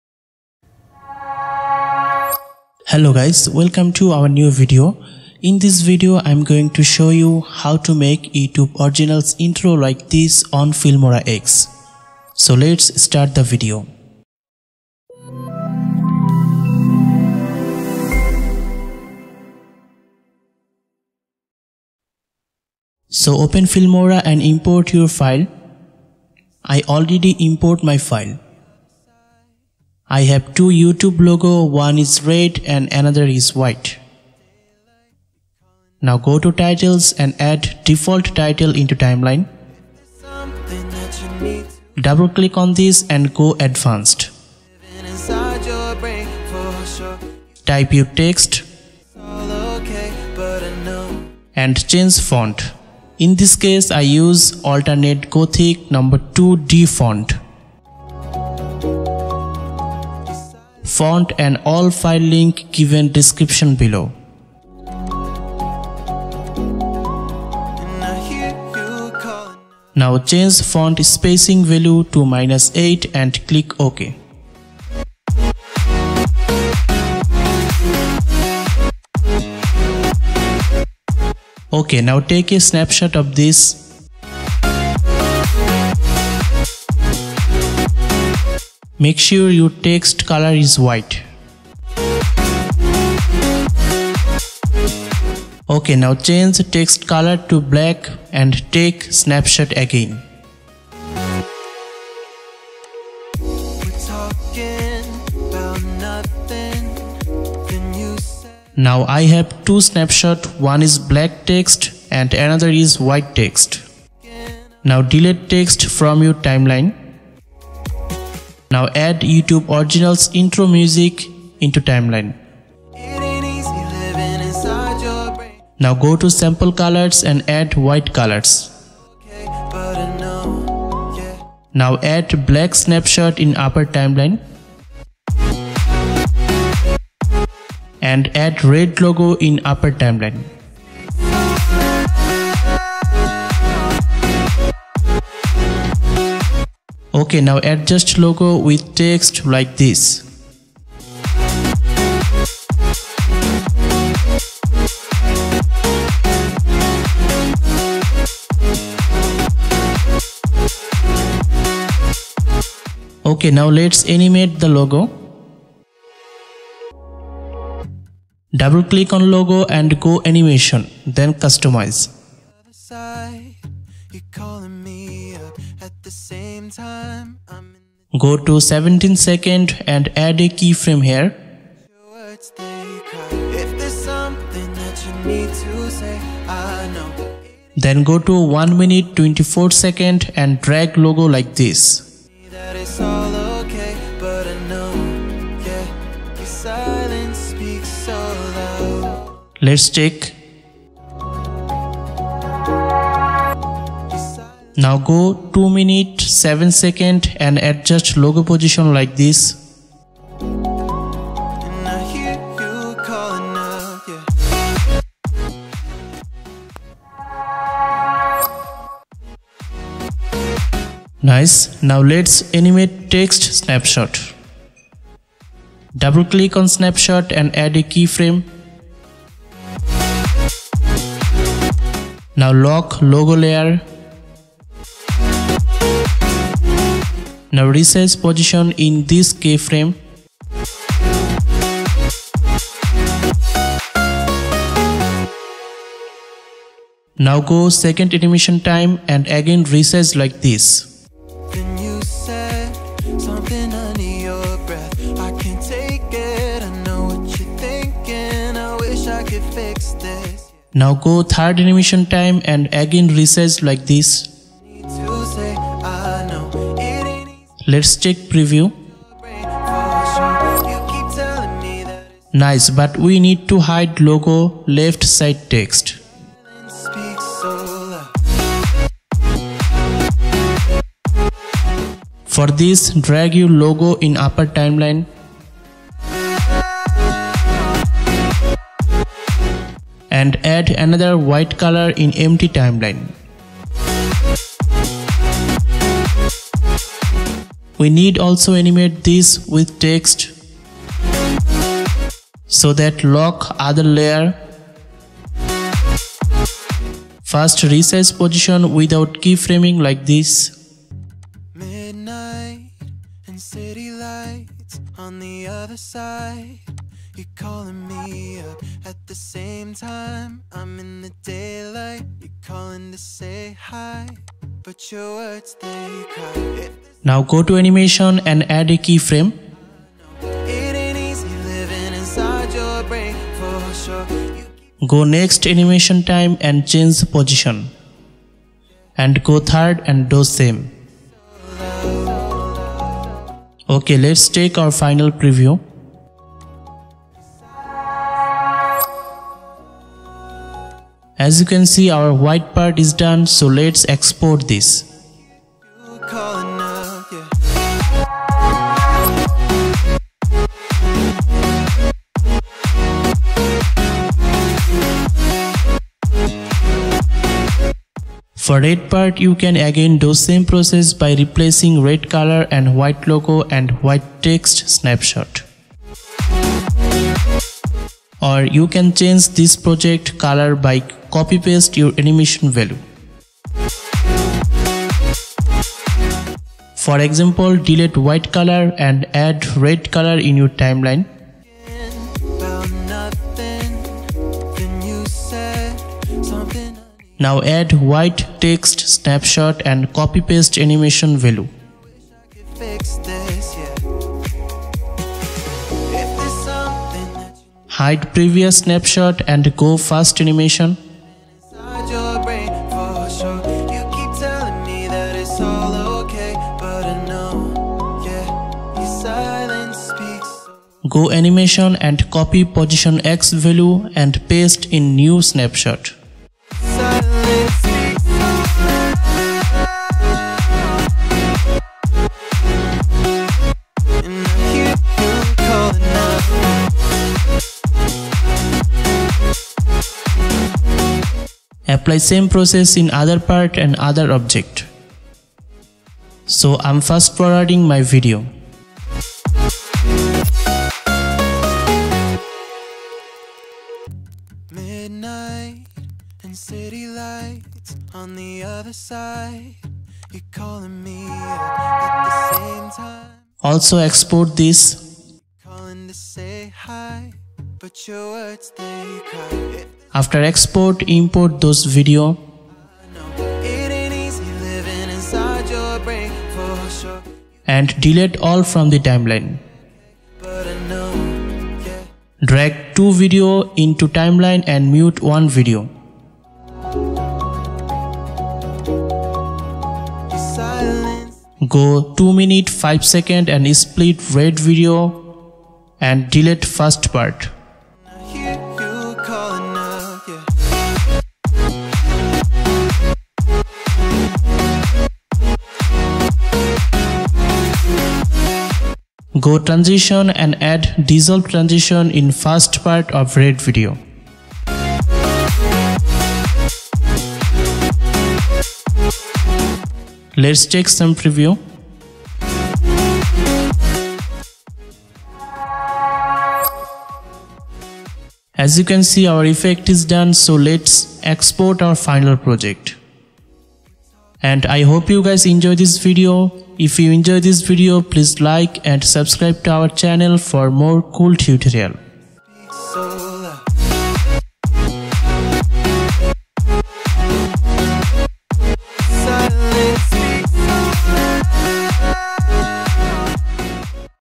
How To Make YouTube Originals Intro On Filmora X | Filmora Intro Tutorial
Note: English Is Not My Mother Tongue. So Sorry If There Is Any Mistake In English

Hello Guys In This Video I'm Going To Show You How To Make YouTube Originals Intro On Filmora X. It's a Very Easy Tutorial. You Can Use Your Text As Your Intro Instead Of YouTube Originals.

Important Part Of This Video
0:00 Preview
0:02 Introduction
0:14 Intro
0:22 Tutorial